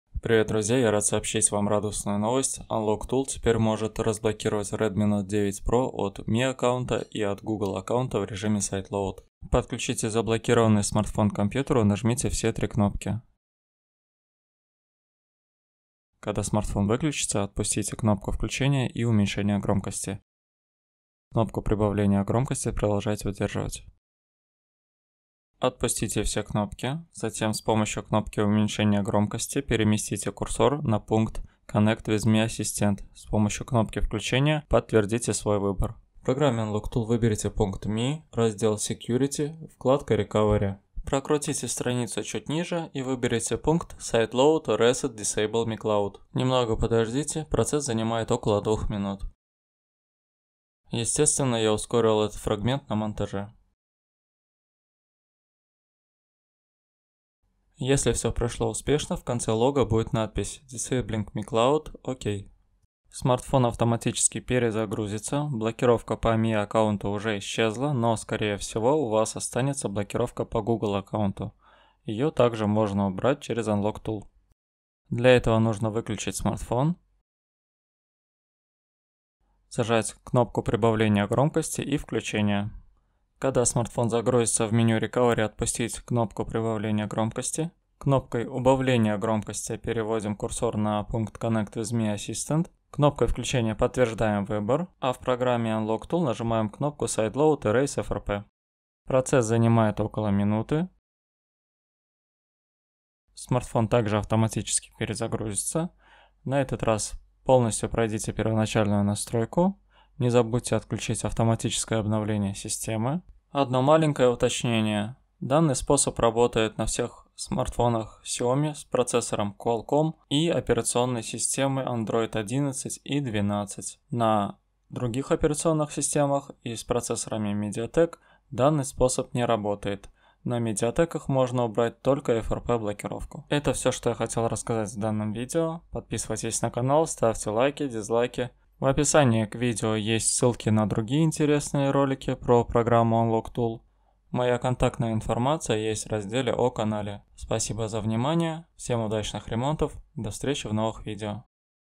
Redmi Note 9 Pro обход Mi аккаунта
Привет, друзья! В этом видео я покажу вам как удалить блокировку по Mi и Google аккаунту на смартфоне Redmi Note 9 Pro с помощью ПЛАТНОЙ программы Unlocktool. Способ работает только если в смартфоне прошивка на базе Android 11 и оболочка Miui 12xxx.
На MIUI13 с Android 12 НЕ РАБОТАЕТ!